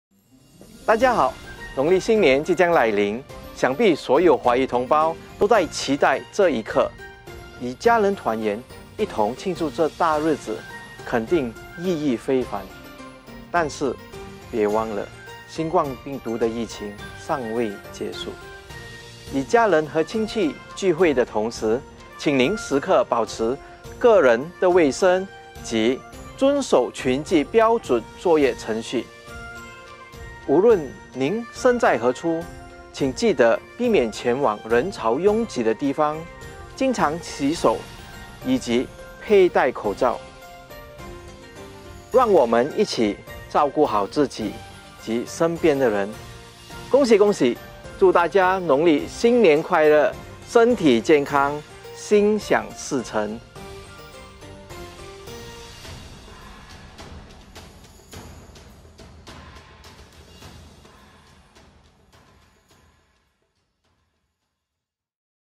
COVID-19 | Sambutan Tahun Baru Cina Di Musim Pandemik
Tahun Baru Cina bakal menjelma. Semua masyarakat cina pasti menantikan detik ini.

Dalam keseronokan berkumpul bersama ahli keluarga dan sanak saudara , pastikan sentiasa mengamalkan kebersihan kendiri dan SOP yang telah ditetapkan.

Sumber : Myhealth KKM